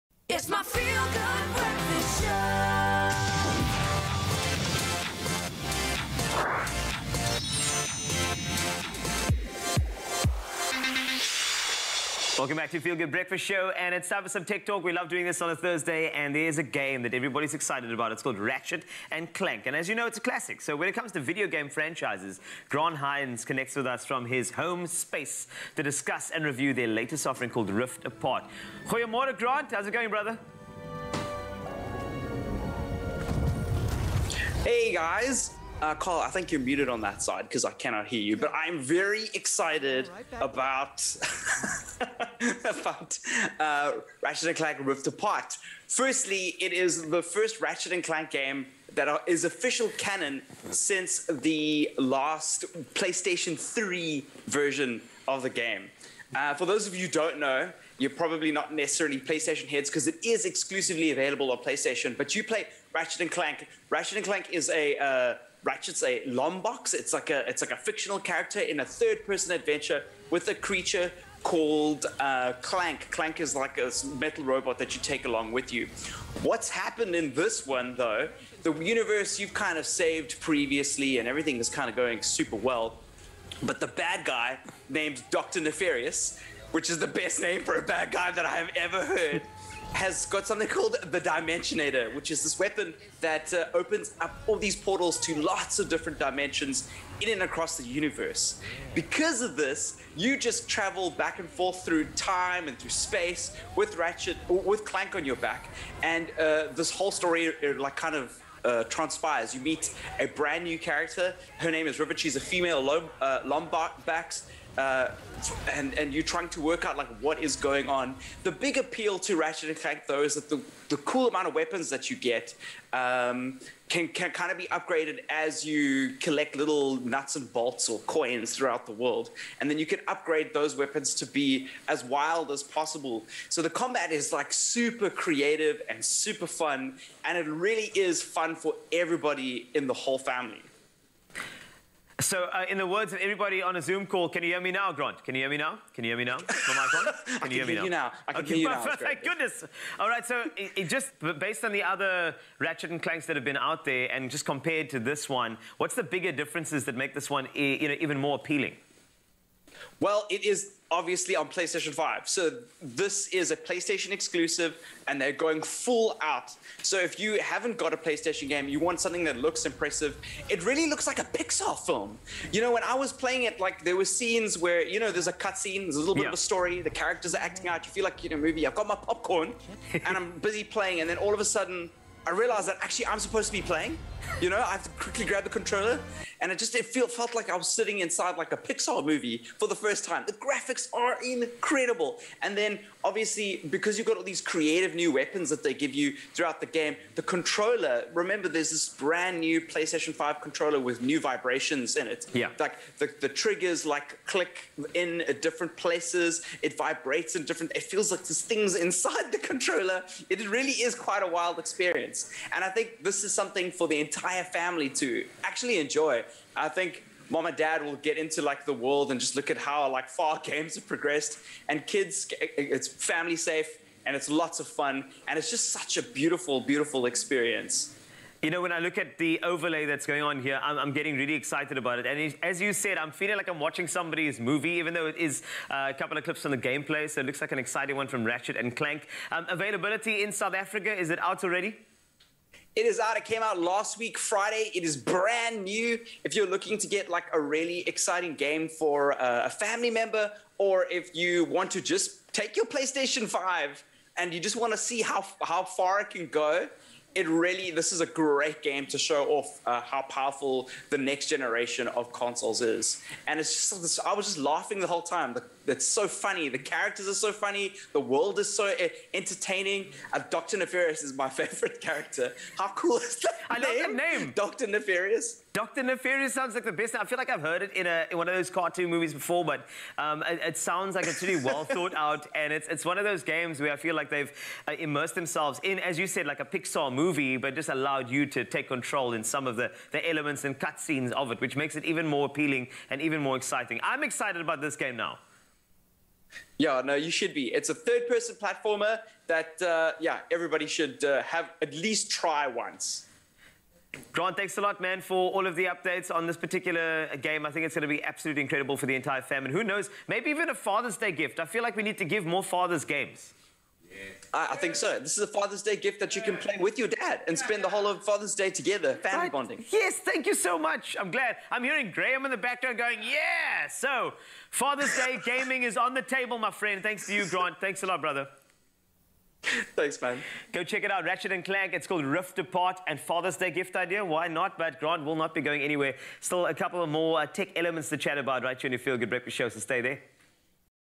Game Review: Ratchet and Clank - Rift Apart
Ratchet and Clank has been a family staple in the world of gaming for many years. Now enters Ratchet and Clank: Rift Apart